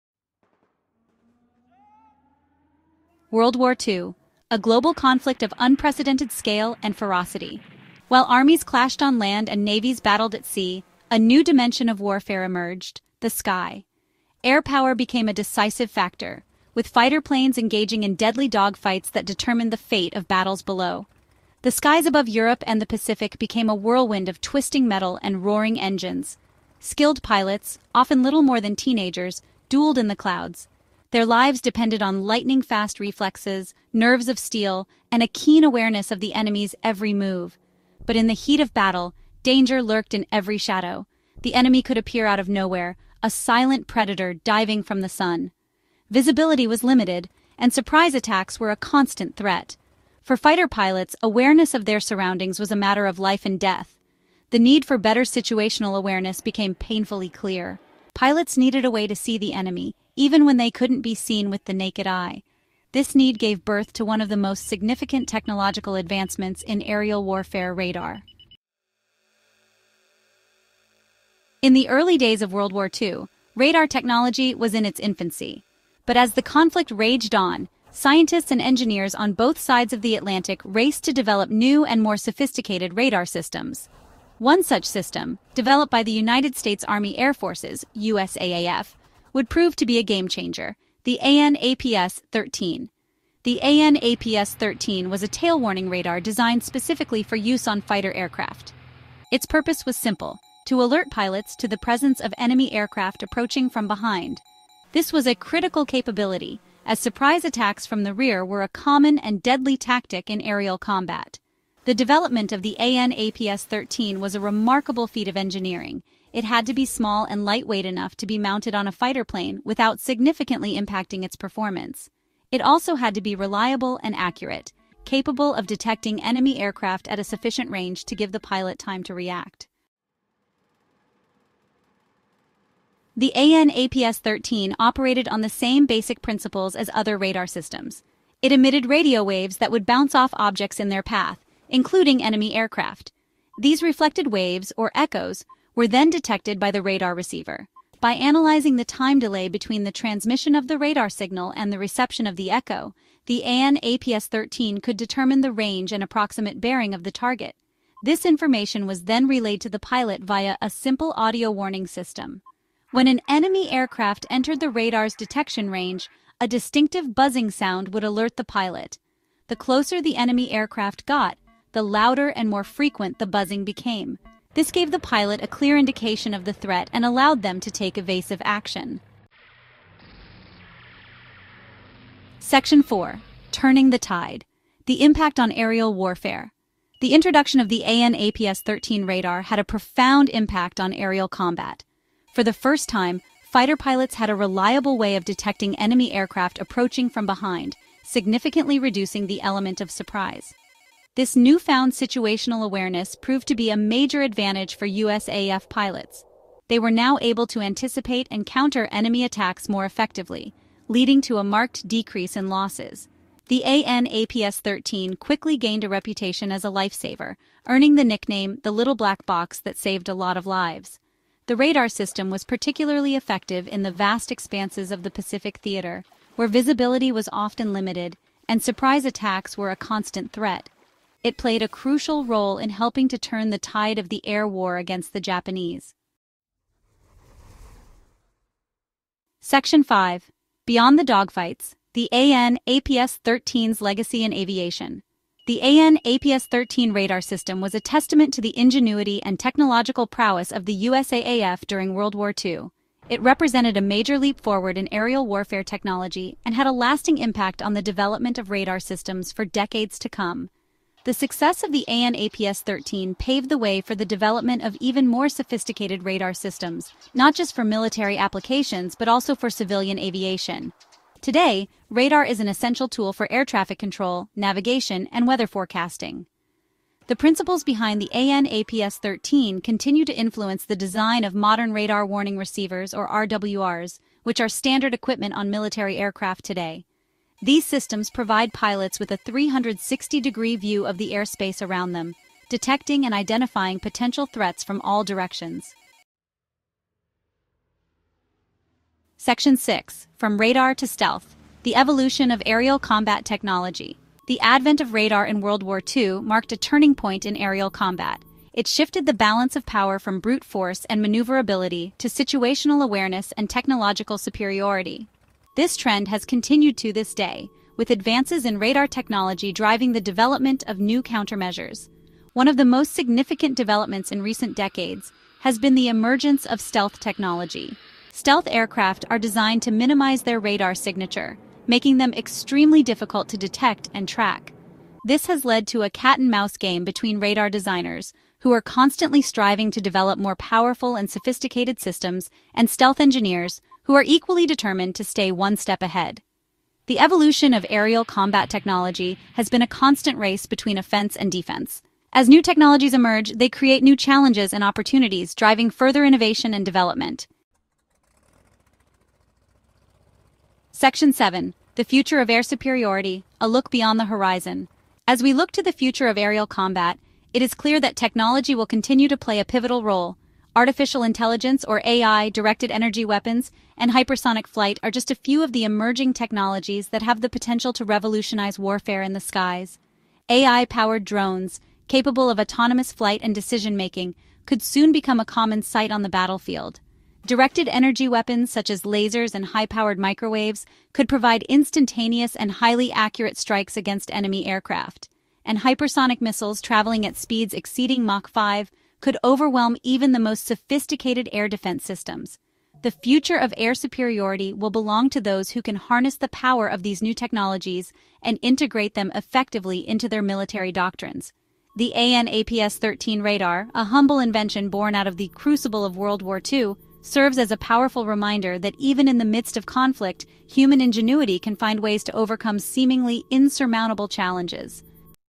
How WWII's AN/APS-13 Radar Revolutionized Aerial Combat!
Explore the groundbreaking impact of the AN/APS-13 tail warning radar, a technological marvel from World War II that transformed aerial combat! In this video, we take you through the intense dogfights of the era and unveil how this radar system provided pilots with critical alerts about incoming threats from behind. Discover the mechanics behind the AN/APS-13, its astonishing ability to reduce fighter losses by 80%, and its lasting legacy that continues to influence modern aviation technology.

Join us as we delve into this fascinating chapter of military history.